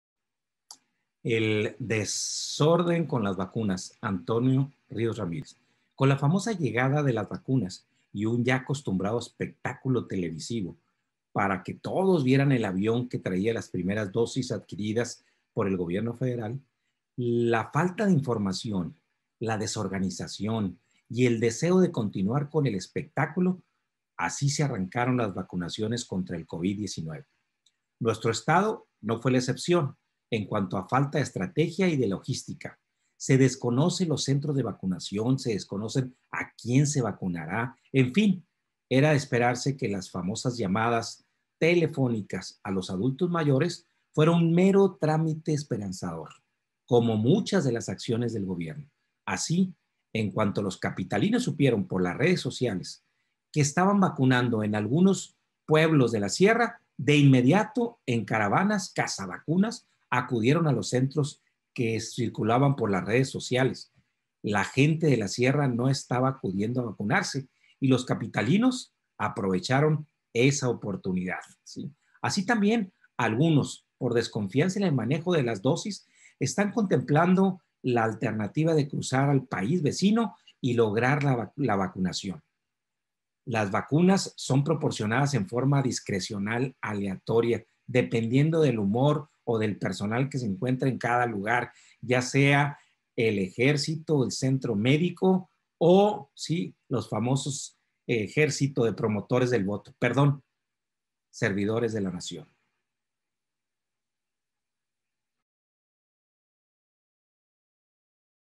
El des….orden con las vacunas | Análisis de Antonio Ríos
Con la famosa llegada de las vacunas y un ya acostumbrado espectaculo televisivo para que todos vieran el avión que traia las primeras dosis adquiridas por el gobierno fedeal, la falta de información, la desorganización y deseos de continuar con el espectaculo, se arrancaron las vacunaciones contra el covid19.

*LAS OPINIONES VERTIDAS EN ESTE CLIP SON RESPONSABILIDAD DEL AUTOR Y NO REPRESENTAN LA POSICIÓN EDITORIAL DE LA OEM*